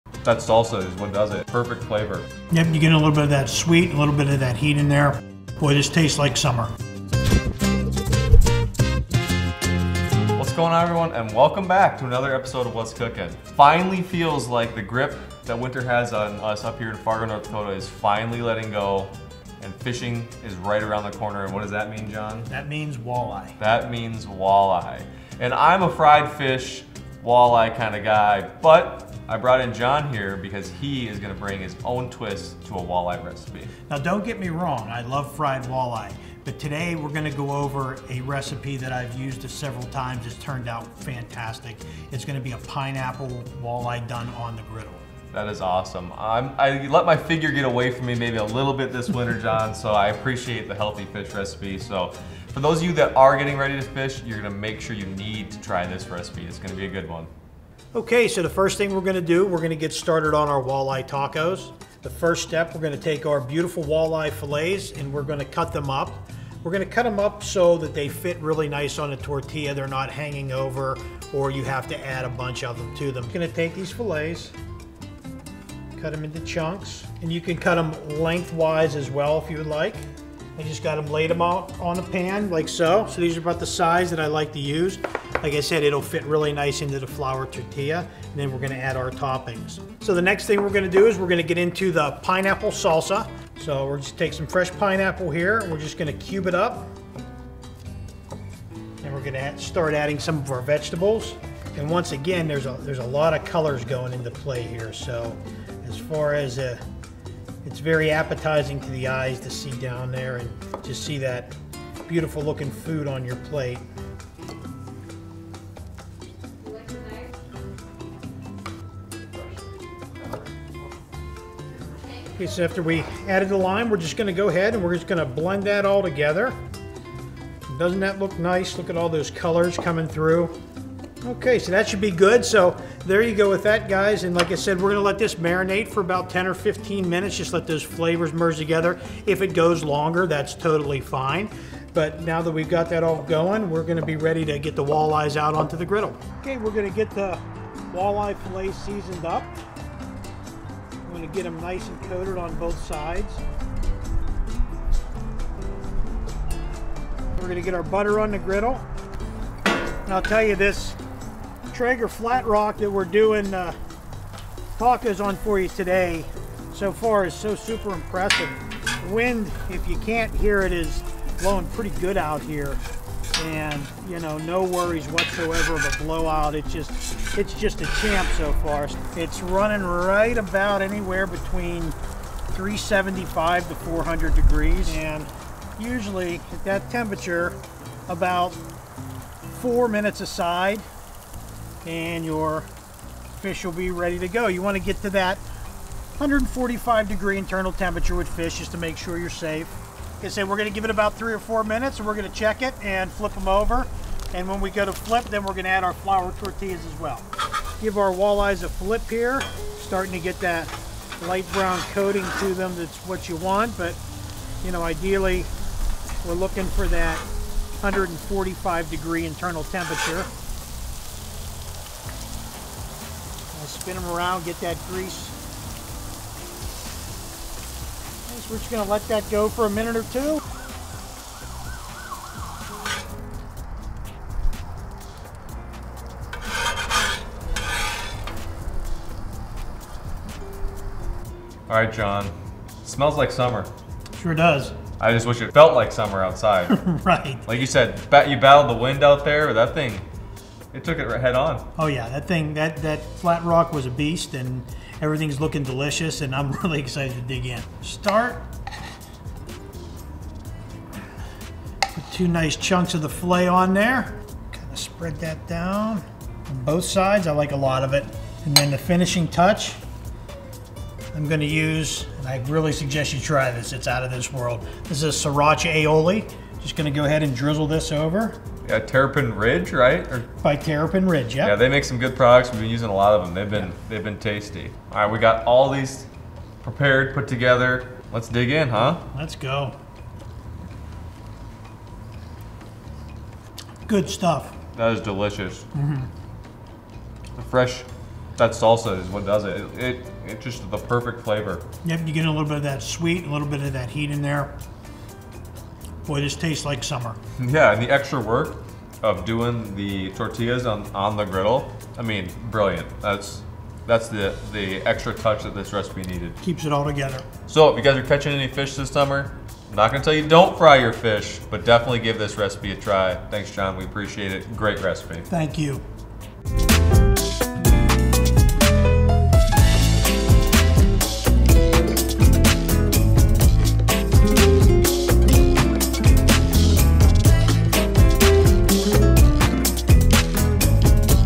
Easy Pineapple Salsa Walleye Tacos Recipe on the Griddle
0:00 Fish Tacos
0:58 Prep your ingredients
2:27 Cook Fish
4:02 Enjoy!

Discover the perfect fusion of flavors in our Pineapple Salsa Walleye Tacos recipe. Watch as we guide you through the steps to create these mouthwatering tacos that combine fresh walleye fish with a tropical twist. Taste the paradise!

About SCHEELS:
SCHEELS is a destination sporting goods store. SCHEELS exists to engage with the outdoor community.